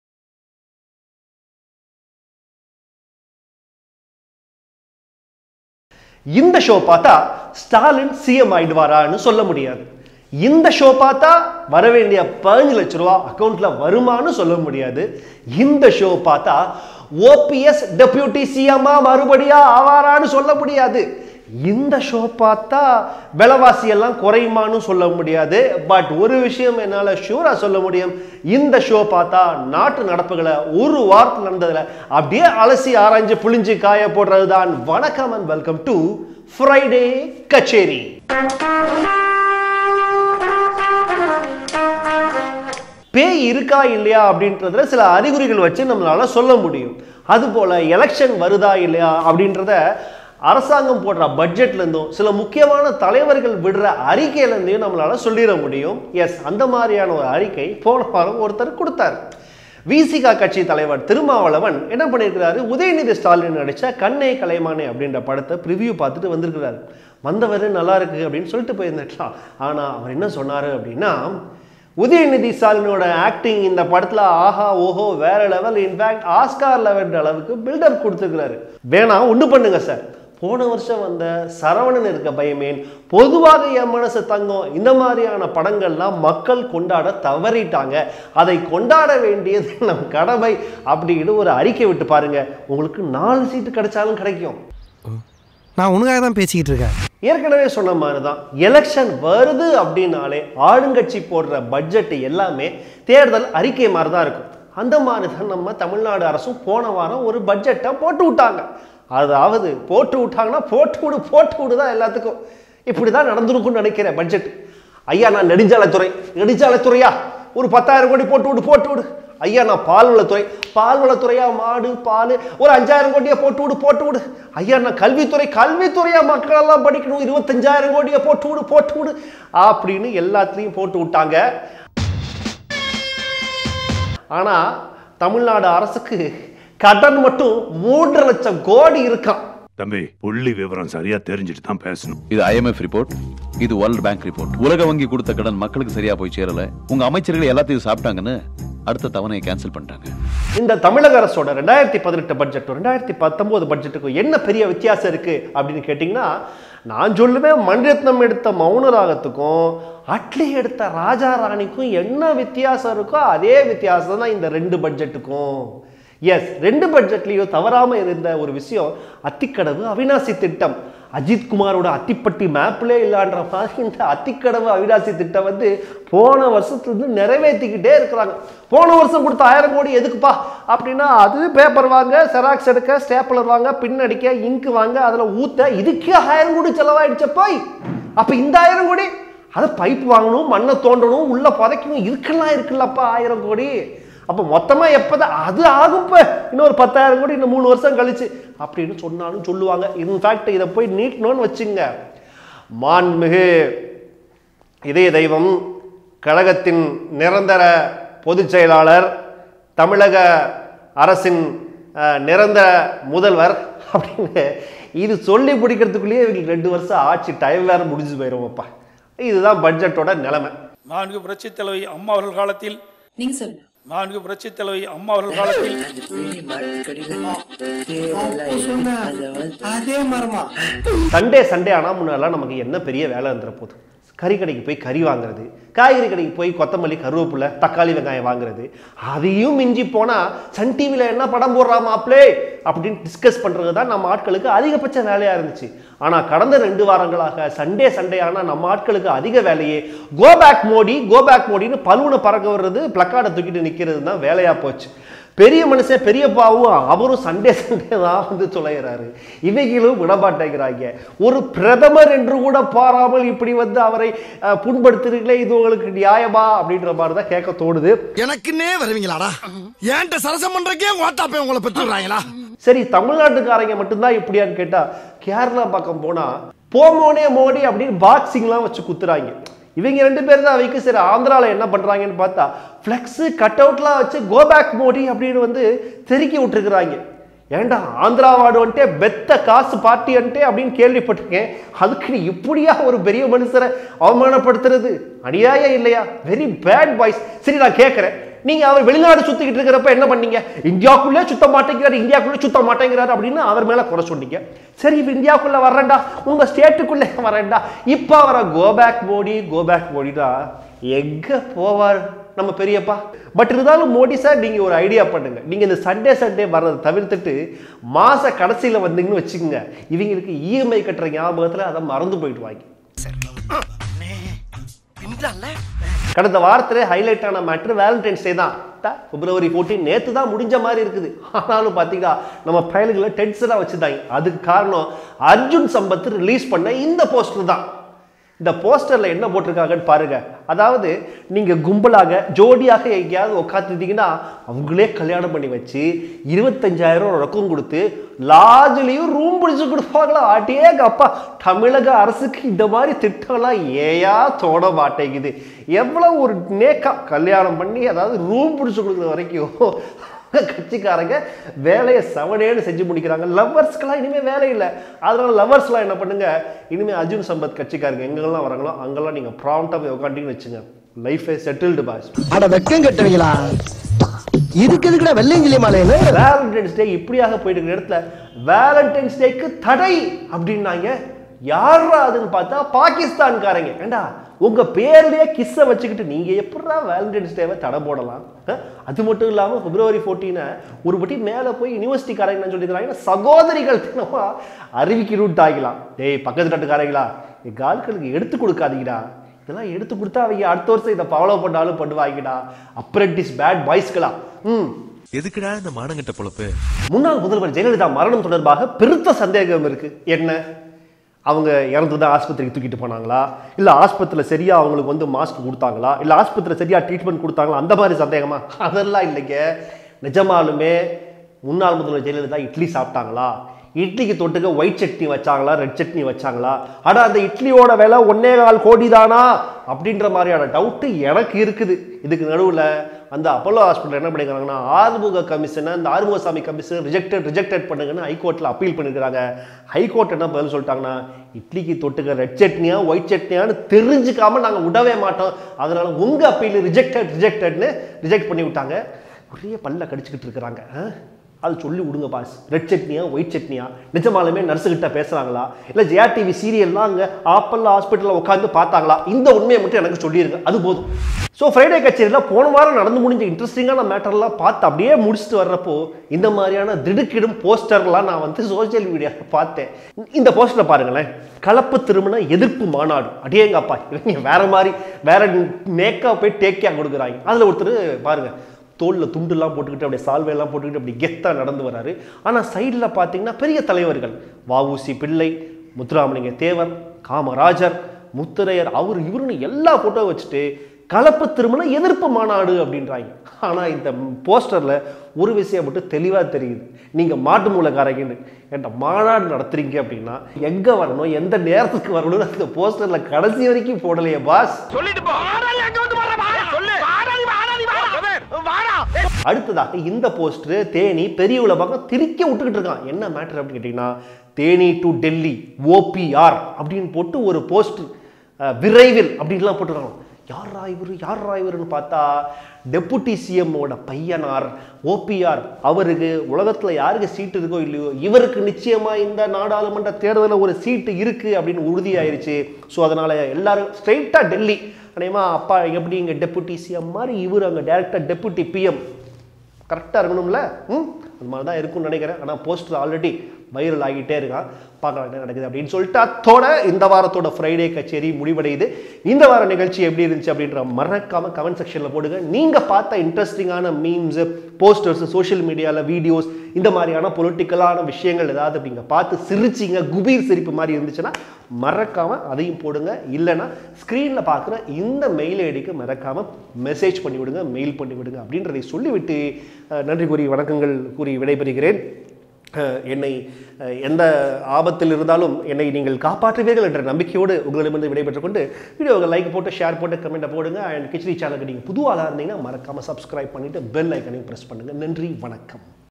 Go Back Go back na எங்க போவார் ? |Fryday Katcheri
In this week's fryday katcheri, we talk about tamilnadu budget, go back modi issue, thiruma, bjp in tamilnadu, udhayanidhi stalin and lot more tamil nadu political issues and news.
Watch all the fun and enjoy